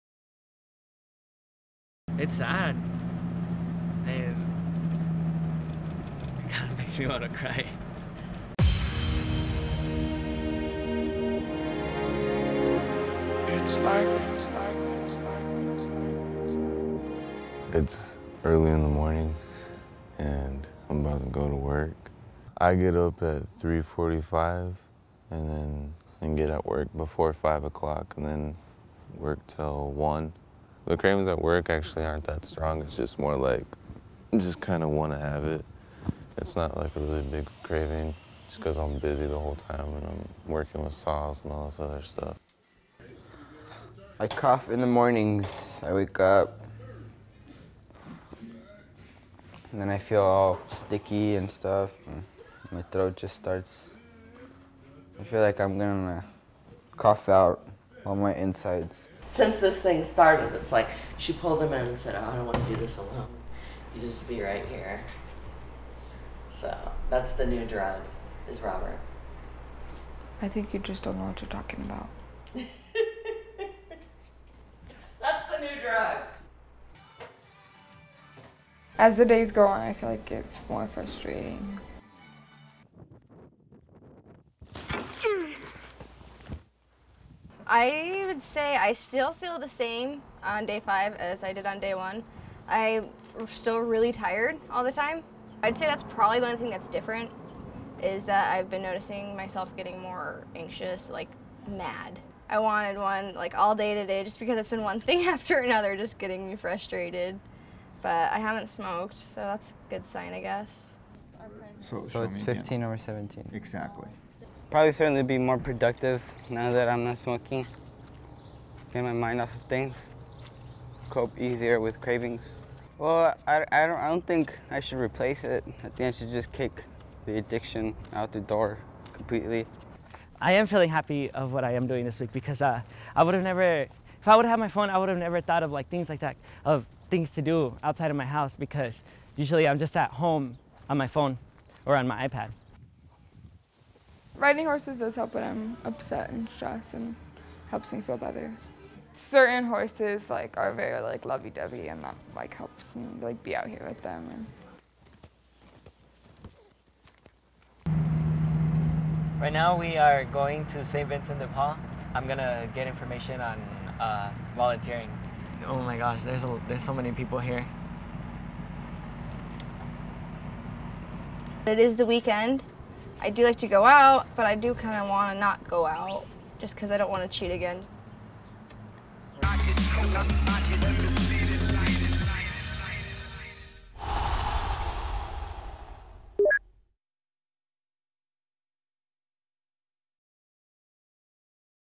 Addicteen Day 5 - Day By Day
Addiction is a physical and mental battle. Our heroes talk about how they're
taking their mind off of cravings. It involves heavy machinery and pizza.